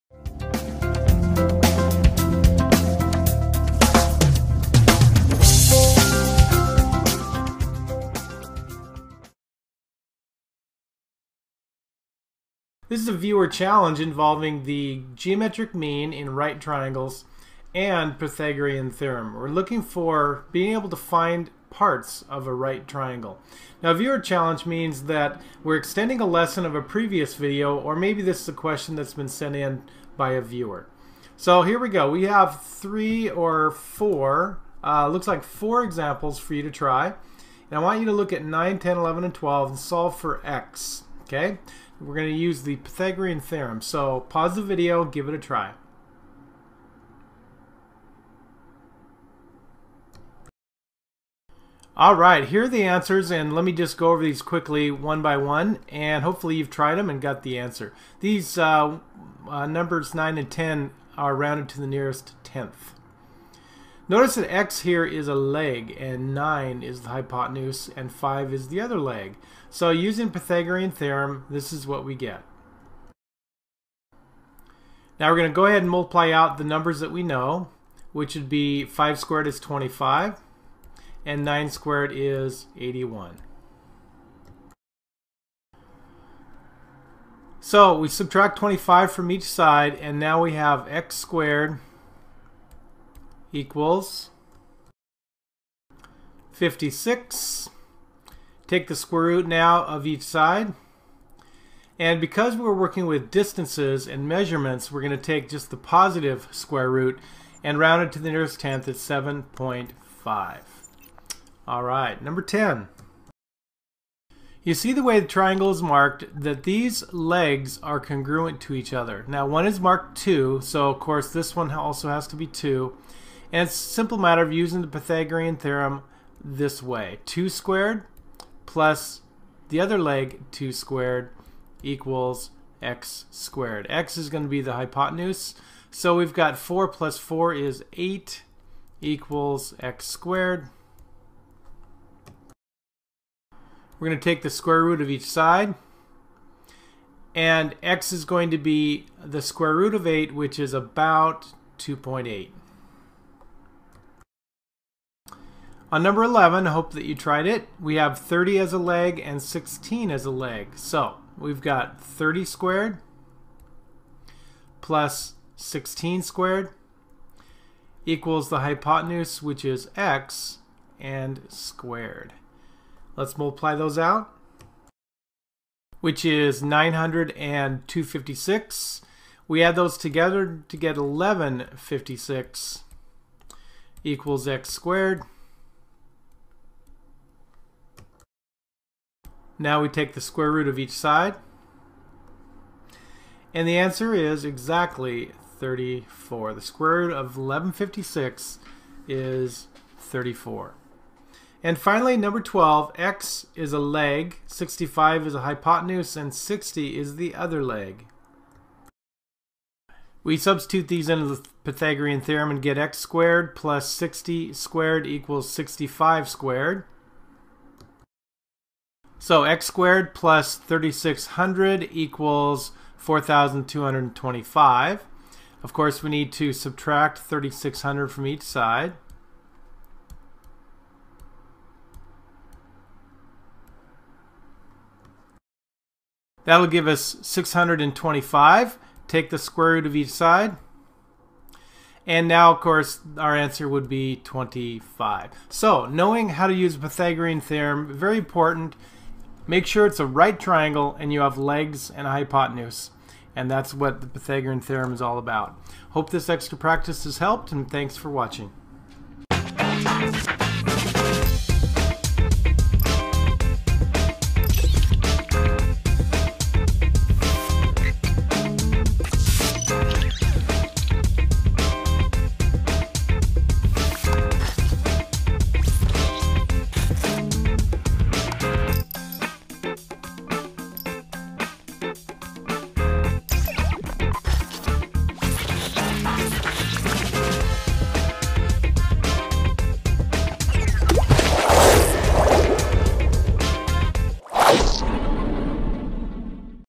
Using Pythagorean Theorem: Missing Sides of Right Triangles
These video examples will have you using the Pythagorean Theorem, finding the missing sides of right triangles.